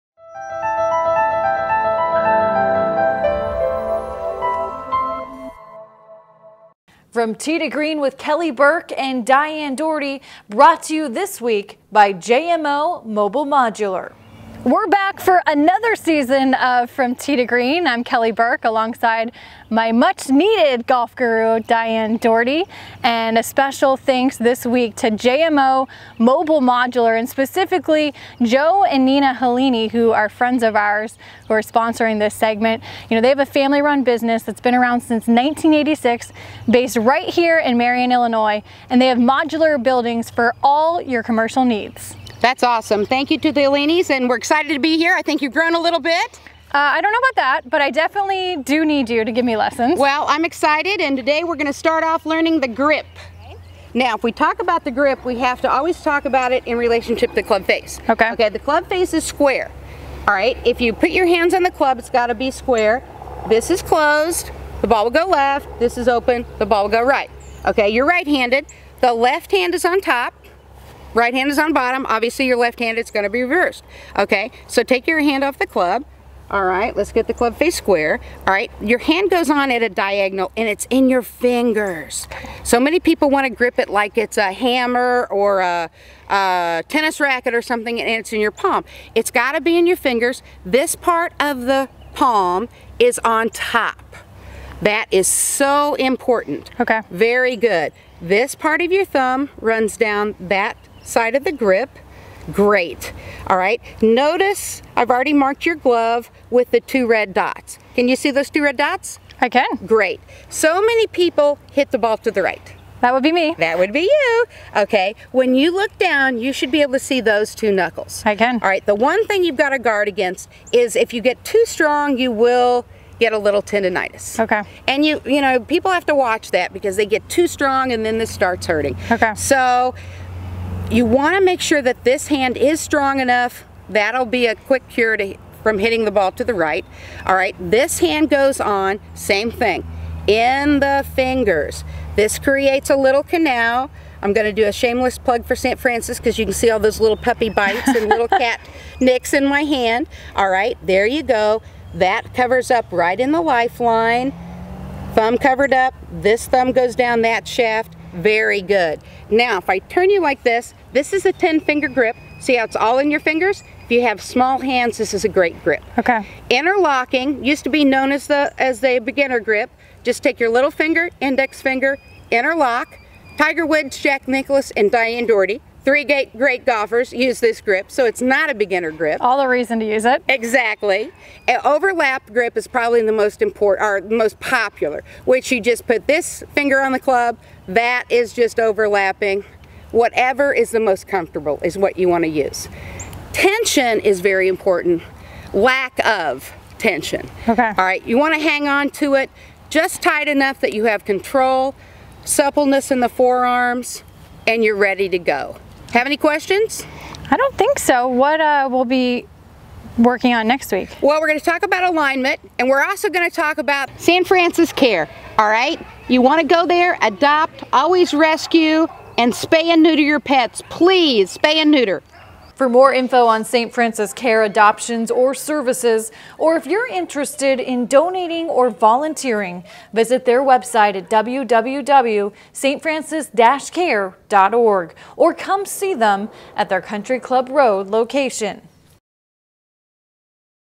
From Tee to Green- Week 1
On this edition of From Tee to Green, Kelly Burke and golf pro Diane Daugherty deep dive into the 'grip' and how by changing your grip, you can hit the ball straighter.